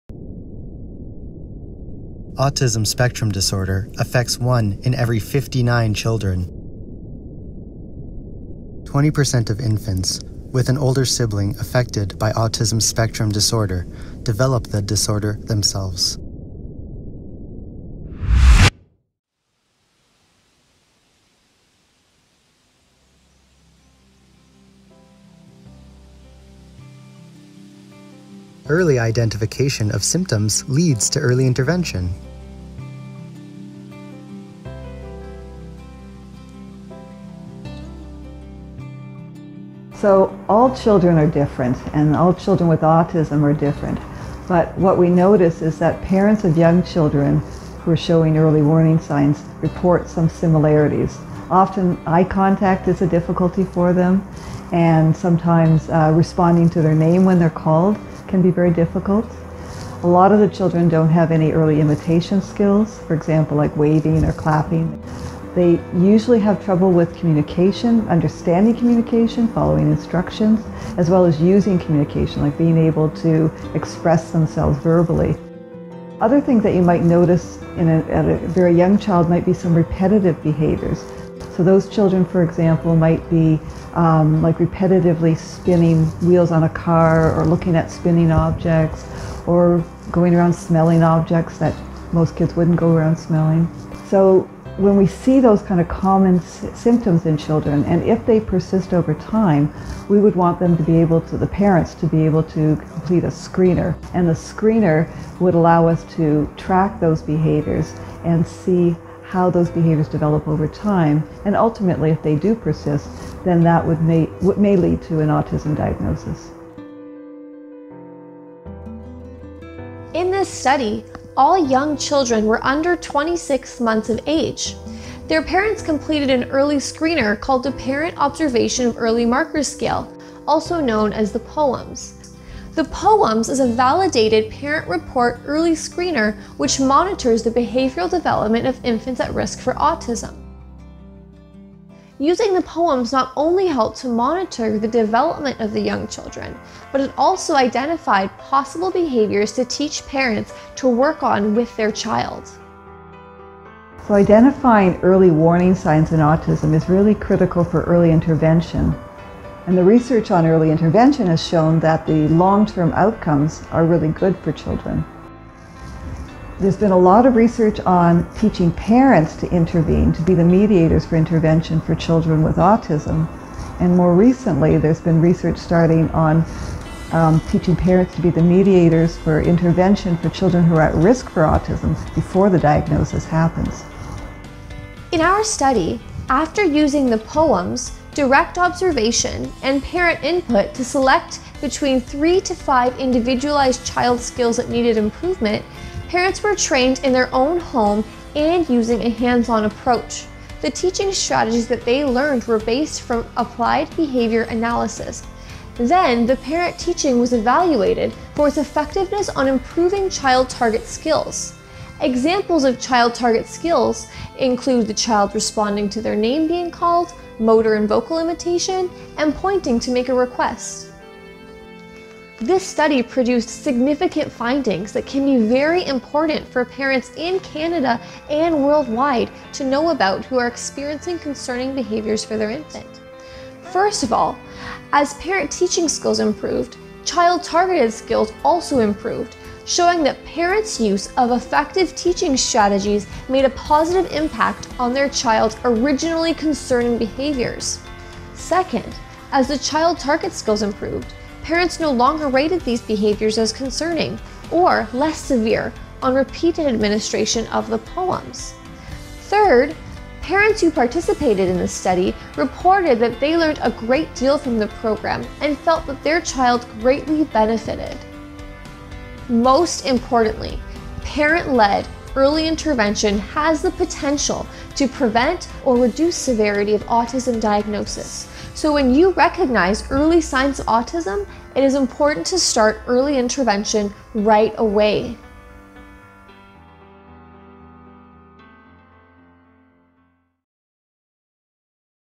Parent-mediated targeted intervention for young children at-risk for Autism Spectrum Disorder
Alicia Azzano, Brock University